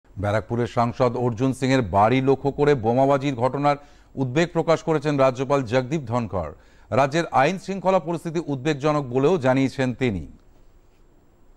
রাজ্যে আইনশৃঙ্খলার পরিস্থিতি উদ্বেগজনক - রাজ্যপাল
রাজ্যে আইনশৃঙ্খলার পরিস্থিতি উদ্বেগজনক, সাংসদ অর্জুন সিং-এর বাড়িতে বোমাবাজির ঘটনায় রাজ্যপাল জগদীপ ধনখড়ের ট্যুইট।